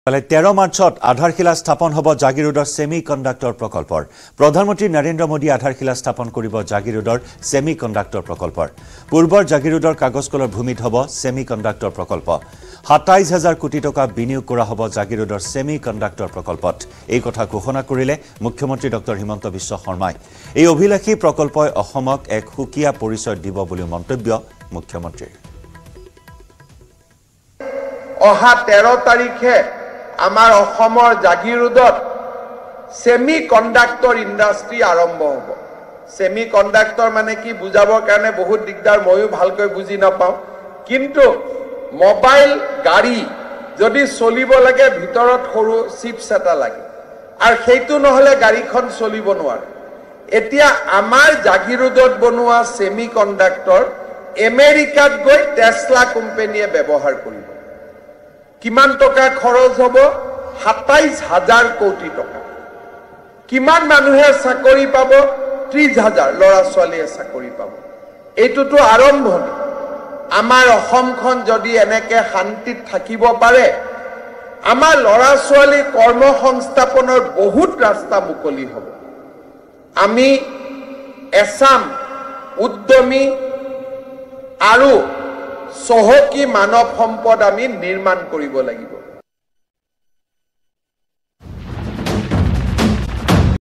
PM to Inaugurate Jagiroad Semi Conductor Project | মোদীয়ে স্থাপন কৰিব জাগীৰোডৰ ছেমিকণ্ডাক্টৰ প্ৰকল্প
PM to Inaugurate Jagiroad Semi Conductor Project |  ১৩ মাৰ্চত আধাৰশিলা স্থাপন হ’ব জাগীৰোডৰ ছেমিকণ্ডাক্টৰ প্ৰকল্প। প্ৰধানমন্ত্ৰী নৰেন্দ্ৰ মোদীয়ে আধাৰশিলা স্থাপন কৰিব জাগীৰোডৰ ছেমিকণ্ডাক্টৰ প্ৰকল্প। পূৰ্বৰ জাগীৰোডৰ কাগজ কলৰ ভূমিত হ’ব ছেমিকণ্ডাক্টৰ প্ৰকল্প। ২৭,০০০ কোটি টকা বিনিয়োগ কৰা হ’ব জাগীৰোডৰ ছেমিকণ্ডাক্টৰ প্ৰকল্পত। এই কথা ঘোষণা কৰিলে মুখ্যমন্ত্ৰী ড০ হিমন্ত বিশ্ব শৰ্মাই। এই অভিলাসী প্ৰকল্পই অসমক এক সুকীয়া পৰিচয় দিব বুলি মন্তব্য মুখ্যমন্ত্ৰীৰ।